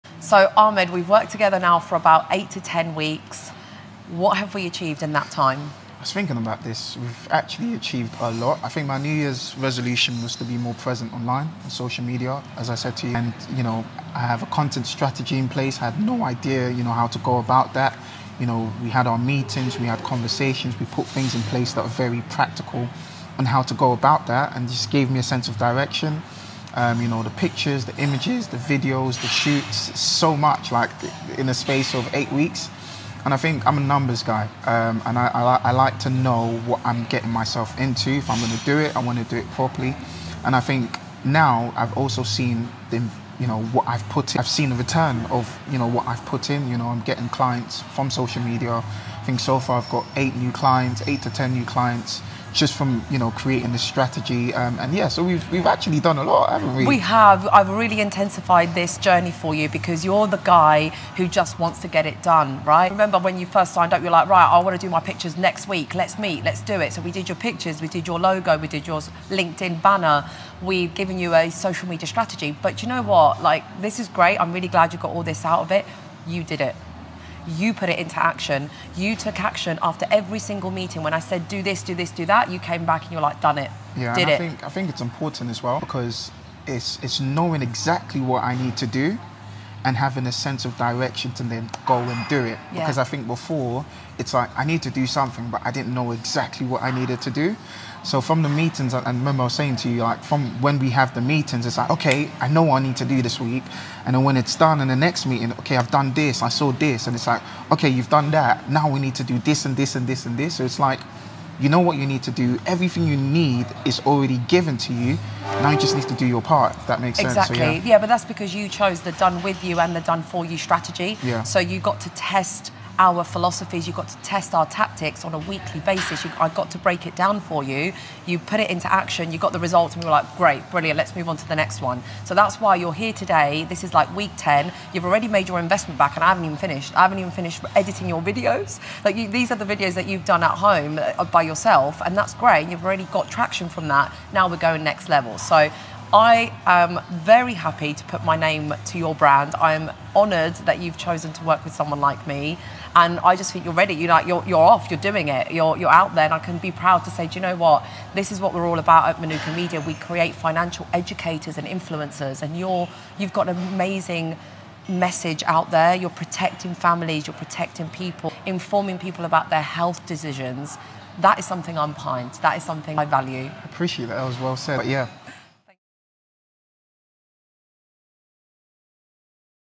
Organic lead generation on Instagram and LinkedIn
Protection adviser Ahmed from AIB Protect has already made back more than his investment with social media and content marketing!

After 8 weeks of working together, he's already made back his investment and then some!

At the time of recording this video we counted 8 new clients! Not leads... actual enquiries which turned into paying clients.

Ahmed has been on a very fast track service with us at Manuka Media going through the following in a very short space of time:

1. As he trades under Precise Protect he didn't have his own brand or identity.

We created a fresh logo for his own brand as a trading style of Precise Protect.

He thought of the name and we brought it to life. We gave him a logo, colour palette and fonts!

2. We designed a LinkedIn banner for his profile with his mission statement

3. Ahmed has never had any personal pictures! He didn't have any photos of himself so we booked a photoshoot to get pictures ready for social media, the press, and for his future website.

4. Ahmed had never ever posted on Instagram prior to working with us! It was empty. We showed him how to do stories, reels and grid posts and now he's off!

5. We brainstormed endless content ideas and how to find them. I taught him how to make posts more personal and how to elevate your brand message

6. We designed Canva templates for him and video thumbnails

7. Ahmed attended one of our video filming days too where we delivered 20 videos fully edited for him ready to use.

Ahmed now has the confidence to post professional and personal posts with the satisfaction of getting results on social media!

He's a very busy guy so we had to move very fast for him and we are over the moon with how happy he is!

He's made protection advice and insurance very engaging and approachable.

It's been a pleasure to work with you Ahmed, thanks for choosing us